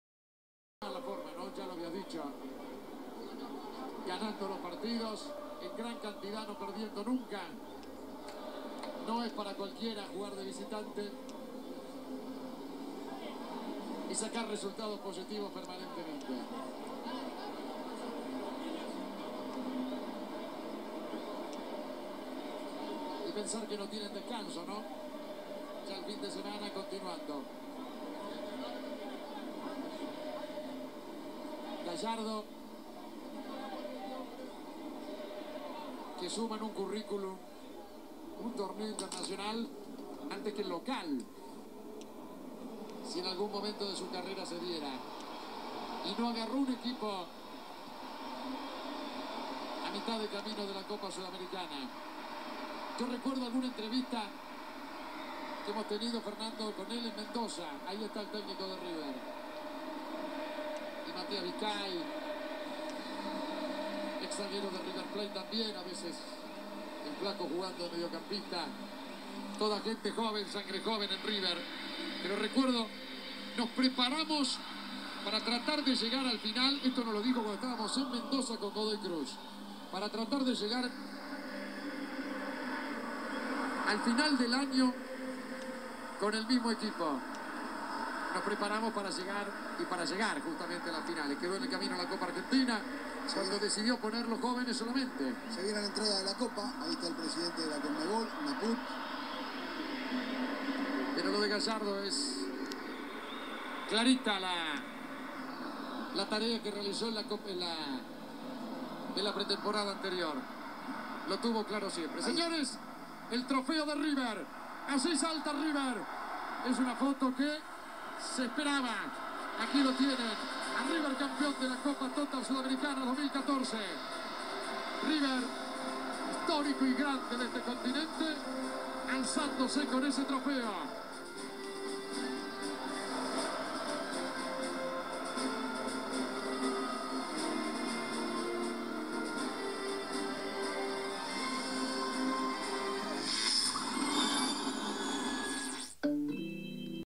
premiacion river plate campeon copa sudamericana 2014
pequeño fragmento de la premiacion del ca river plate
disculpen la calidad de video